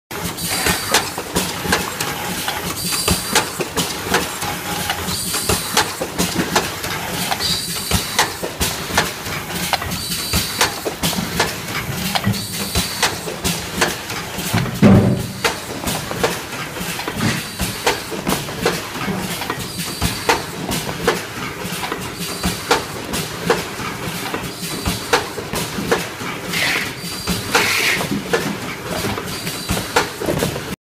Automatic Soap Cellophane Wrapping Machine Soap Cellophane Packing Machine
the cellophane wrapping machine is used for pharmacy, food, healthy products, cosmetics, commodities, stationary and office supplies poker and cigarettes etc.